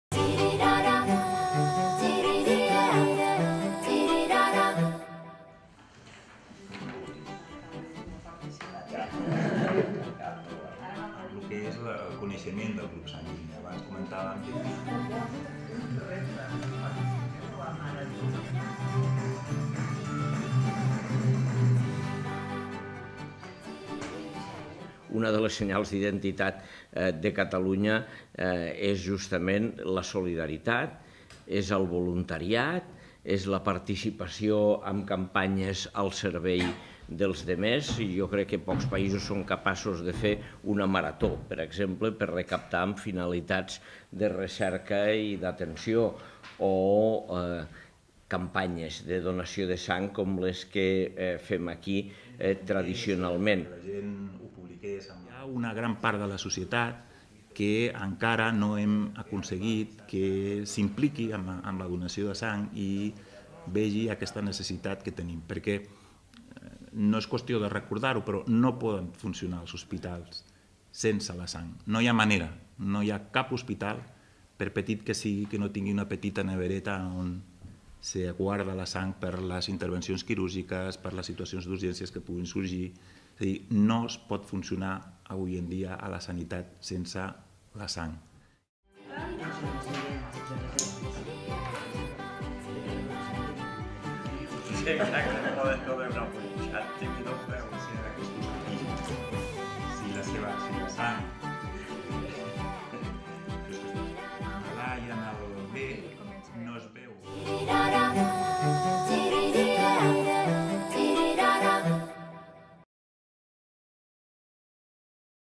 Lleida torna a participar en la Marató de Donants de Sang 2.0
Lleida torna a participar un any més en la Marató de Donants de Sang, que tindrà lloc del 13 al 20 de gener arreu de Catalunya. Aquesta és la campanya de donació més gran, que enguany posa especial atenció per poder incrementar les reserves, després d’unes festes de Nadal en què s’han reduït un 25 % i s’han situat en un nivell baix. El Banc de Sang i Teixits convida a donar sang i a descobrir el grup sanguini, ja que quatre de cada deu catalans no saben quin és el seu. L’alcalde de Lleida, Àngel Ros, amb el director del BST a Lleida, Juan Manuel Sánchez, ha presentat la campanya a la Paeria, espai municipal que el divendres dia 13 estarà obert a la donació d'11 a 14 h i de 16 a 20 h, juntament als ajuntaments de Tarragona i Girona i, enguany, per primer cop el de Barcelona i el Palau de la Generalitat –també el dissabte 14-.

El paer en cap ha afirmat que Catalunya es caracteritza per molts símbols, sent la solidaritat un dels que conformen la seva personalitat. Ros ha assegurat que un dels senyals d’identitat del país és la solidaritat, el voluntariat i la participació en campanyes al servei dels demés, com és aquesta de la marató de sang. Per això, ha agraït la professionalitat i el treball que fan els organitzadors i el conjunt de voluntaris que hi participen.

El doctor Sánchez ha explicat que els donants de sang reben una analítica amb el grup sanguini, un fet que enguany s’ha remarcat per motivar la ciutadania a donar sang i s’impliqui en la campanya. Sánchez ha recordat que els hospitals no poden funcionar sense reserves de sang pel que és important que s’incrementin les donacions. Segons ha indicat, els grups sanguines amb menys reserves acostumen a ser els negatius.

La Marató s’allararà fins al 20 de gener als principals hospitals de Catalunya, com l’Hospital Universitari Arnau de Vilanova de Lleida. En total, s’esperen aconseguir 8.000 donacions de sang que permetin remuntar les reserves de sang, que han baixat després de les festes nadalenques, pel canvi d’hàbits de la ciutadania, el fre i les malalties, entre altres. El lema de la campanya és “La teva sang diu coses. Comparteix-les” i vol posar en relleu la informació que es pot saber d’una persona a partir del grup sanguini, com ara de qui pot rebre sang, a qui pot donar-ne o quins usos pot tenir la seva sang. Un estudi del Centre d’Estudis d’Opinió ha posat de manifest que un 40% dels catalans no sap quin grup sanguini té. El Banc de Sang vol que tothom conegui el seu grup. Per això, dedica la Marató de Donants de Sang 2.0 de Catalunya als grups sanguinis i a tot el que cal saber-ne.